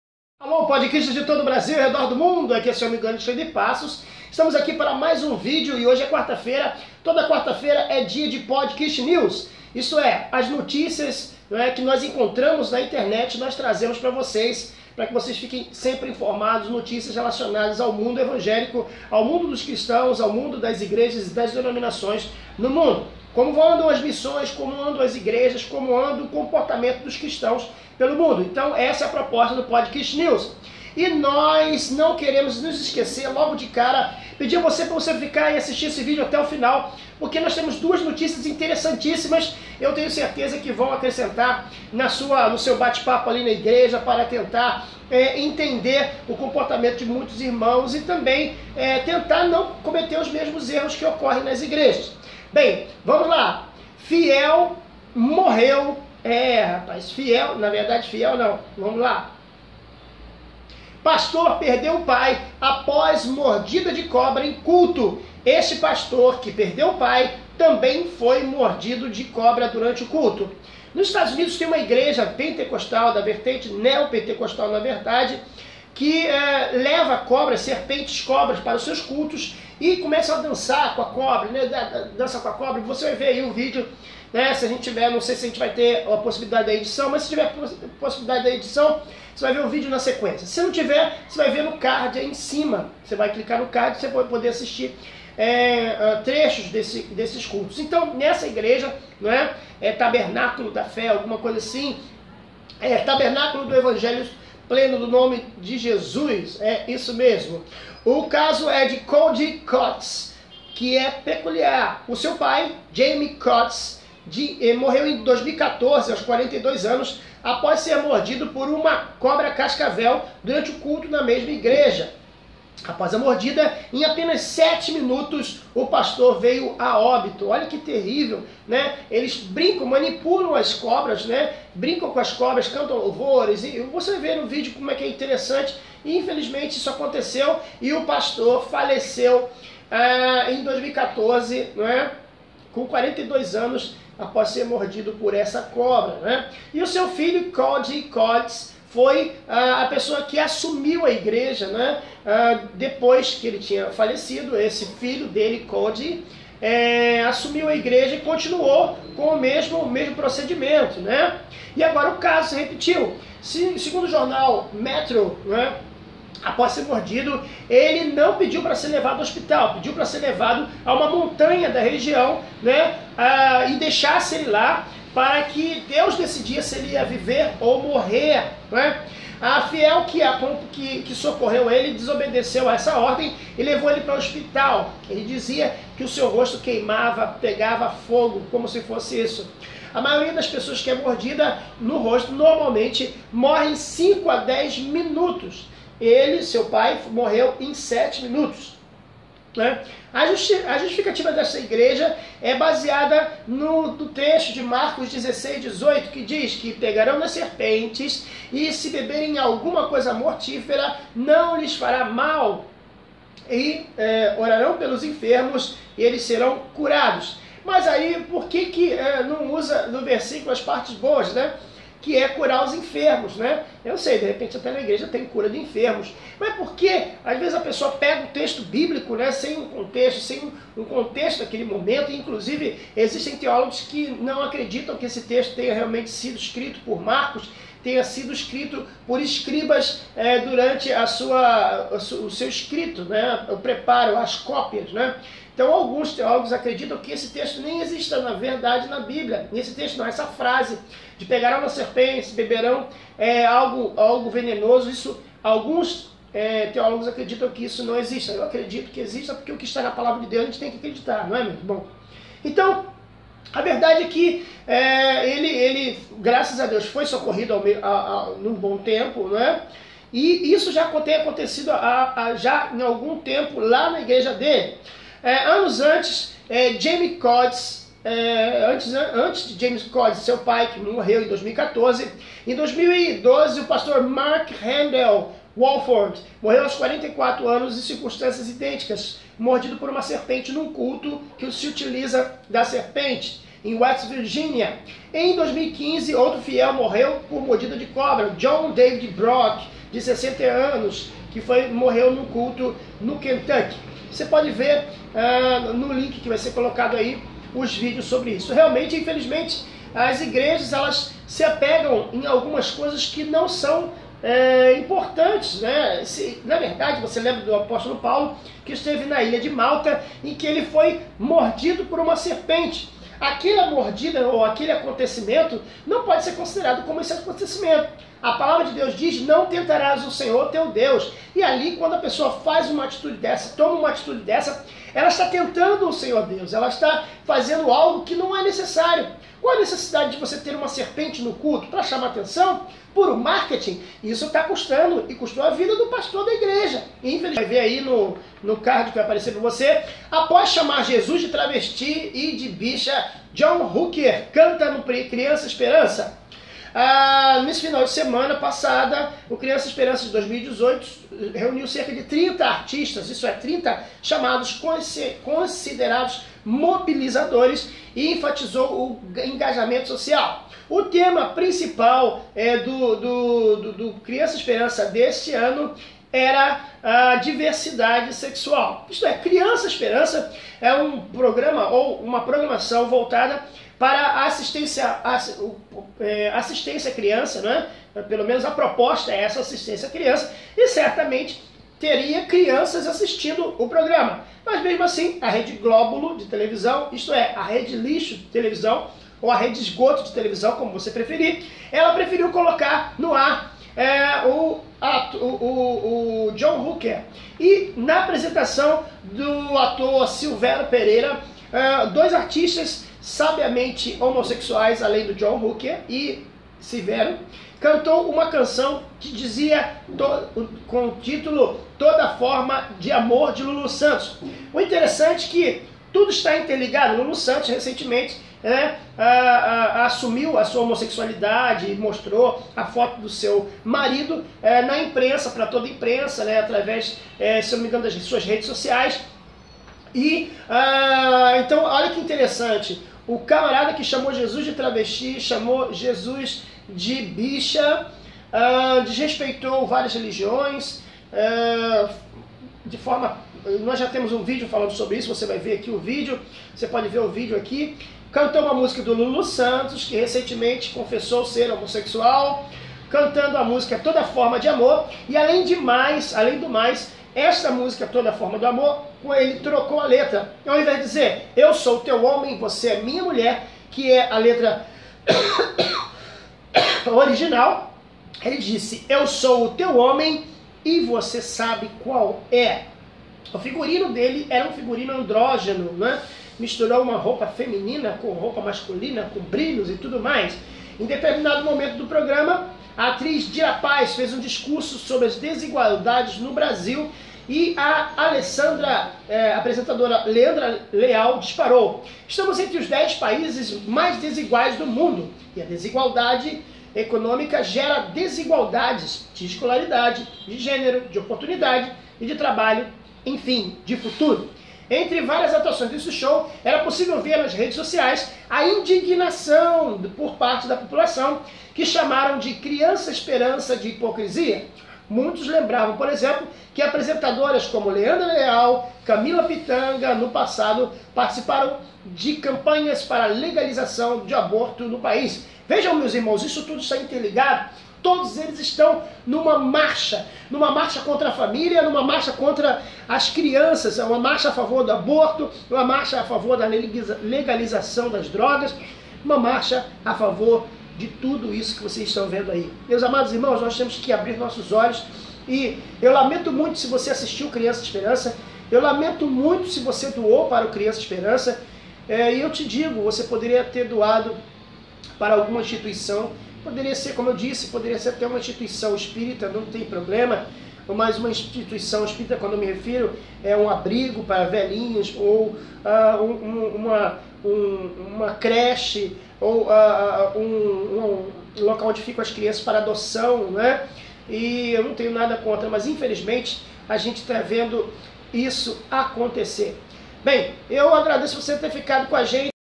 IGREJA DAS SERPENTES - CRIANÇA GAYSPERANÇA II
Neste vídeo falamos de uma igreja muito conhecida nos EUA, que se utiliza de serpentes venenosíssimas, seu pastor morreu em 2014 com uma picada de uma cascavel no rosto, esta prática está
 em outras denominações Neopentecostais da América do norte, no vídeo comentamos esta prática desnecessária.
Outro assunto abordado é a participação e a propagação da "Diversidade Sexual para a causa Criança Esperança" como já falado em Vídeo anterior o "artista" que Chamou Jesus de Travesti, de Bicha subiu no palco do programa que foi ao ar para as famílias dos brasileiros, é a patifaria ao vivo para crianças , Adolescentes e jovens do Brasil, Lamentável.

CONqHEÇA O PODCRIST:
PELA RÁDIO:

link para entrar no GRUPO DE DEBATES PODCRIST